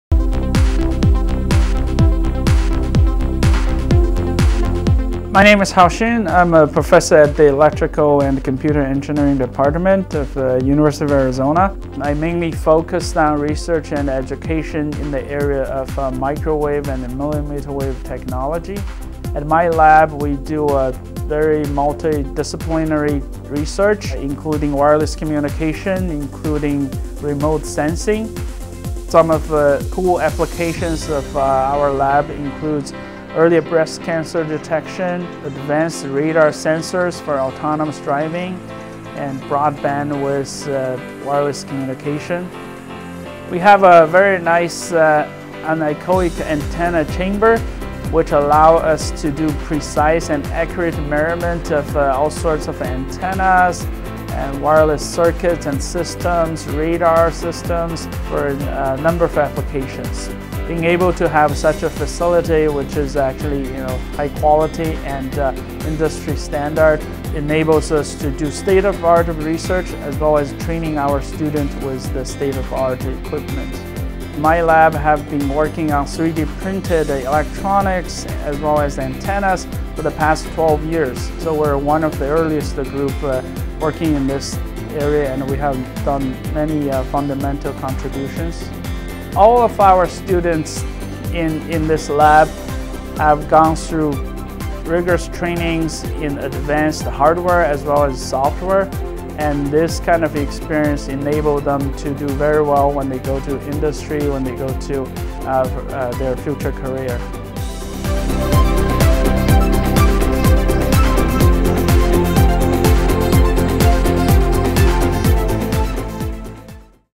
Hao Xin: ECE Faculty Profile
Hao Xin is a professor in the Electrical and Computer Engineering program at the University of Arizona. He talks about his research interests in microwave and millimeter-wave technology and developing advanced radar sensors for autonomous vehicles and wireless communication.